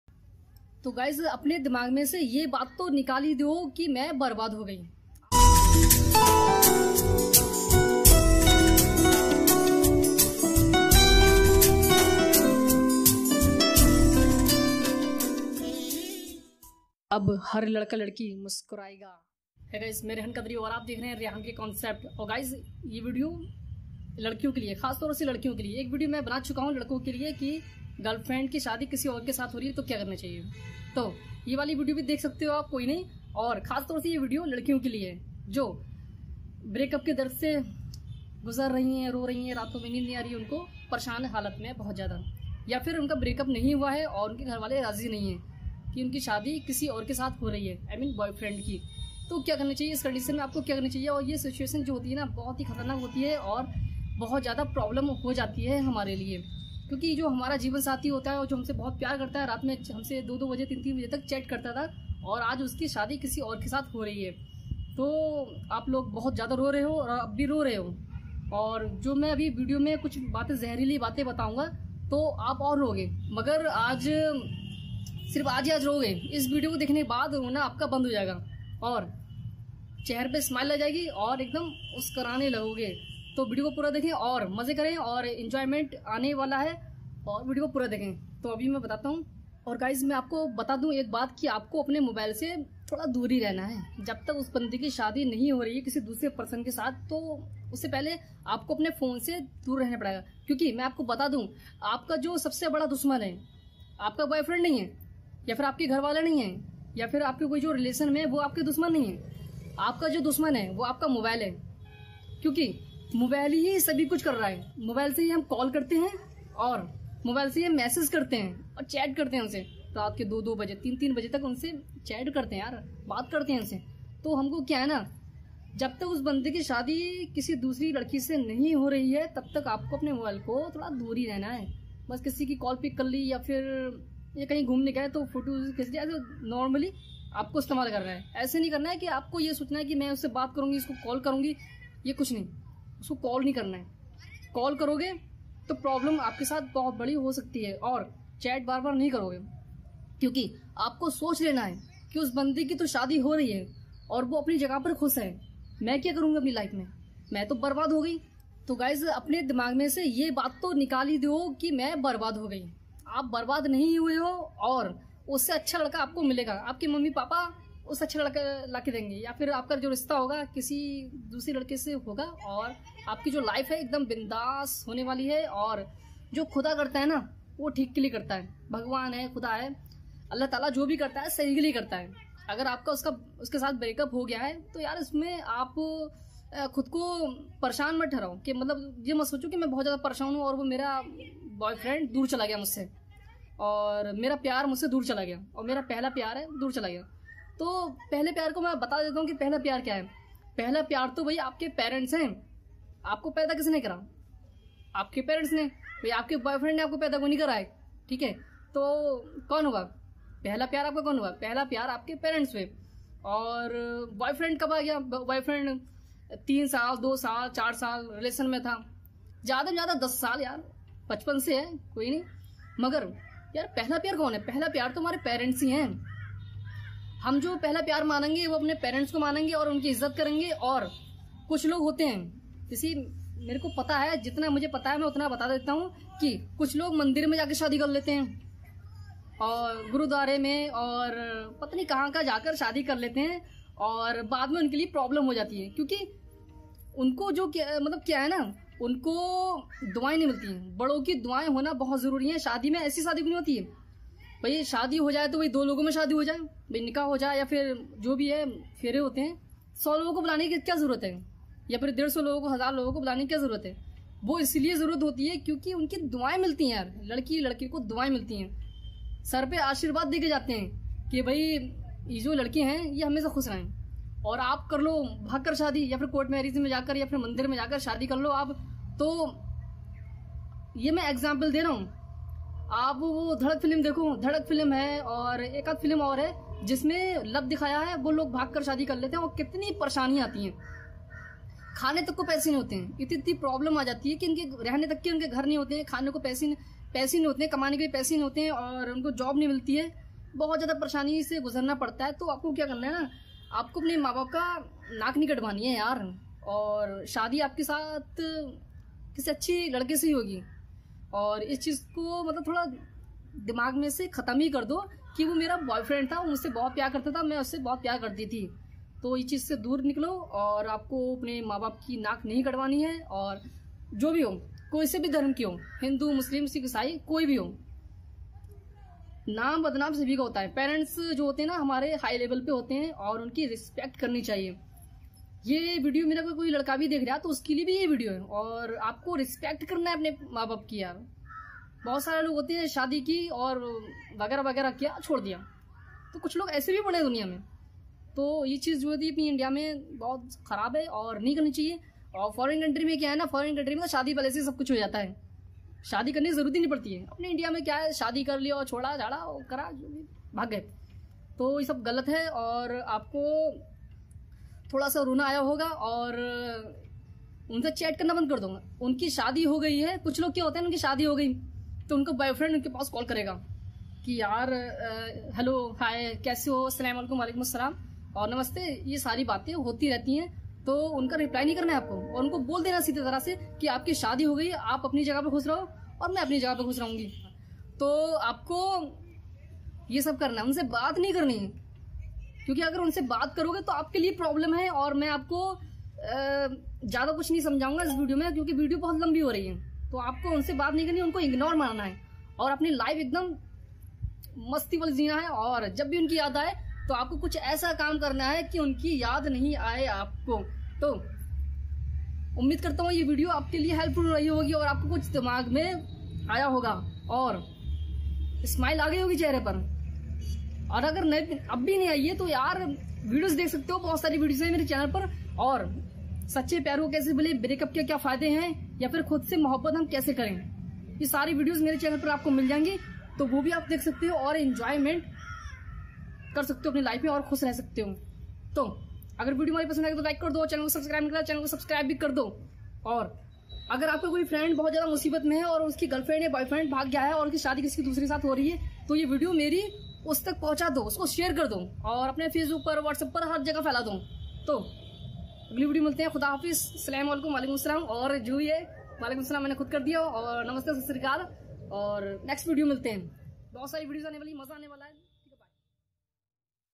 Boyfriend ki shadi kisi doosre se ho rahi hai | Rehan ke concept
Boyfriend ki shadi kisi doosre se ho rahi hai | Rehan Qadri
Hi

HELLO GUYES KAISE HO AAP SAB MAI UMMID KARTA HOO AAP SAB ACHCHE HONGE AUR AAJKI IS VIDEO ME AAPKO YAH PATA CHALEGA KI BOYFRIEND KI SHADI KAHI AUR HO RAHIA HAI TO KYA KARE

is channel par aapko motivational video aur educational video milengi